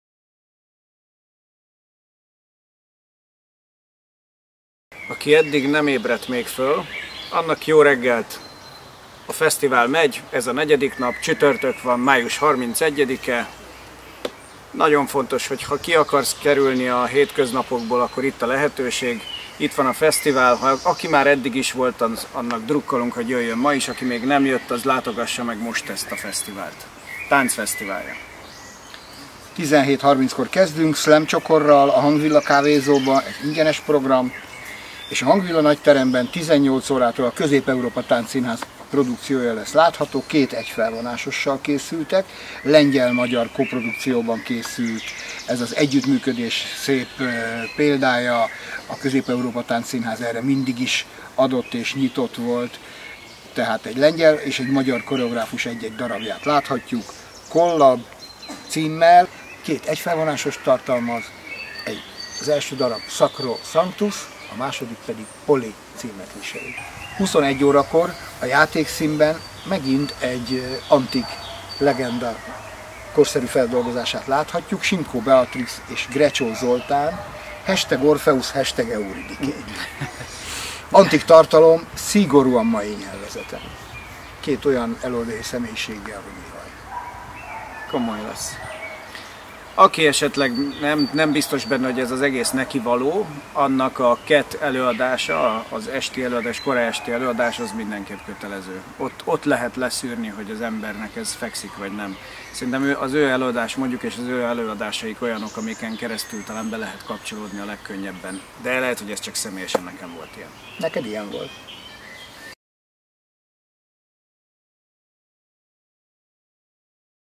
XX. A Tánc Fesztiválja - Veszprém 2018.05.31. 4. nap ajánló
Ha eddig még nem kapcsolódtunk volna be A Tánc Fesztiváljába, most itt a tökéletes lehetőség: a Közép-Európa Táncszínház finom koreográfiája jó belépő a kortárs tánc világába, de adjunk esélyt a műfaj két ikonikus táncosának, Grecsó Zoltánnak és Simkó Beatrixnak is.